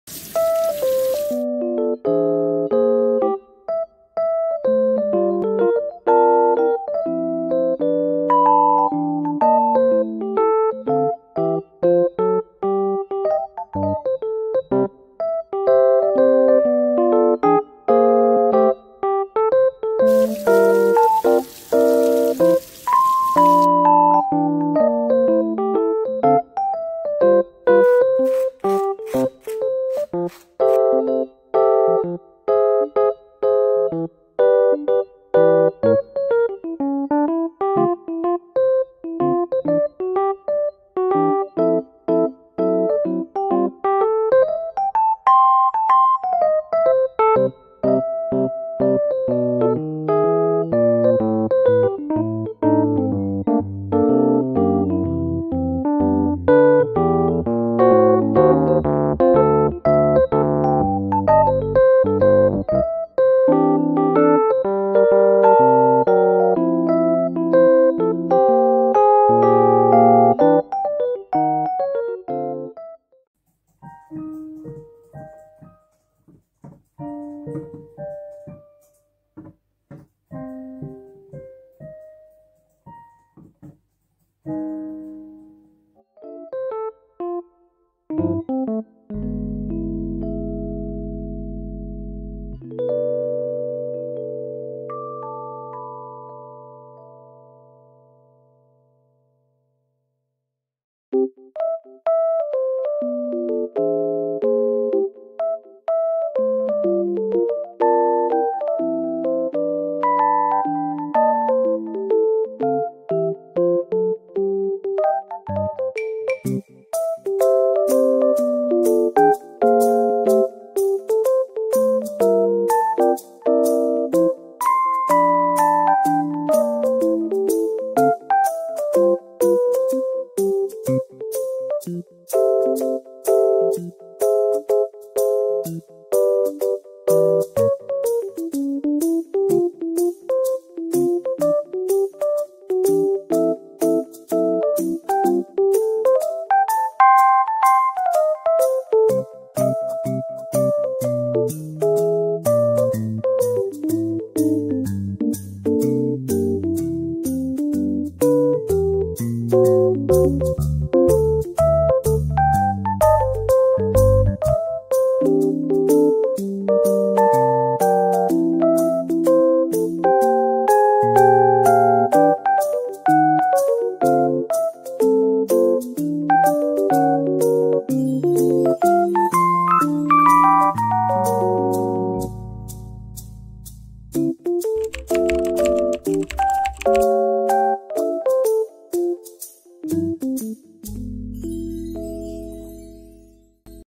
Morning Vlog - Breakfast, Itaewon Class, TATBILB 3, journaling, cute flowers
Hello, in today's video I will be showing you my chill morning routine! I hope you enjoy it!